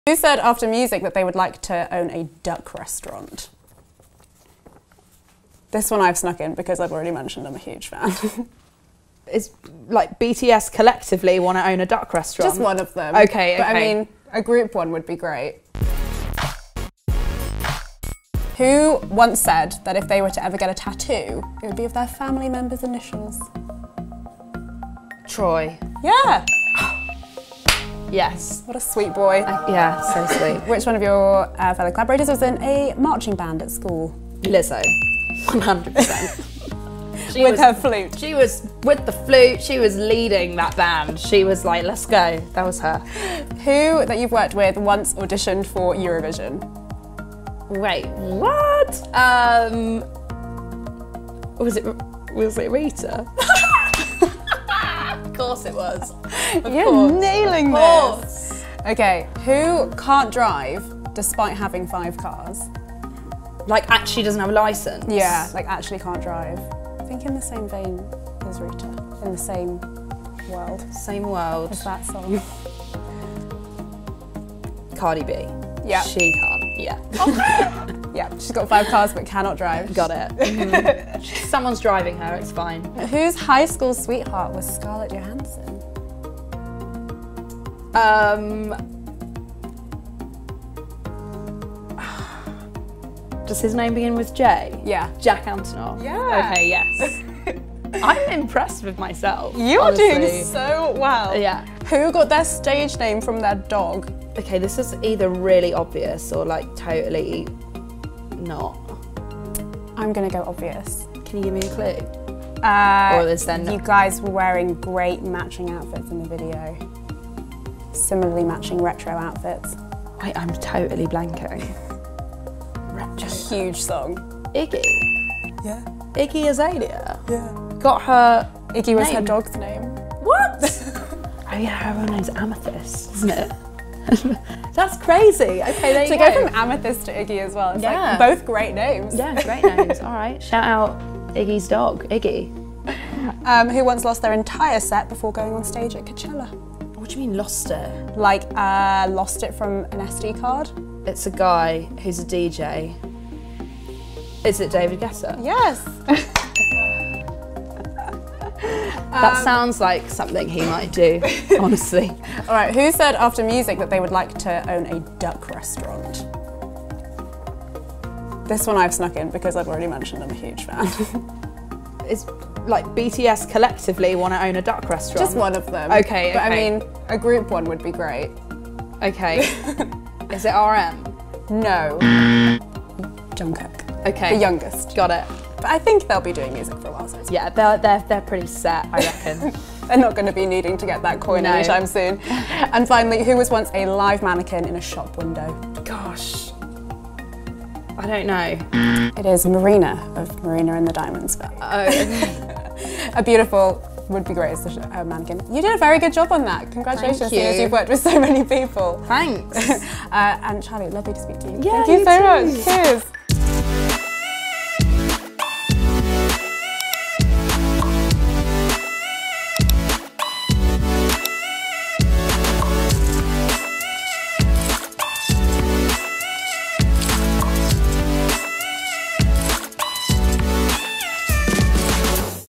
"BTS want to open a duck restaurant?!" Charli XCX takes on The Big Collab Quiz
Charli XCX has worked with some of the hottest artists so we thought, why not put her to the test to see how well she know them 🤔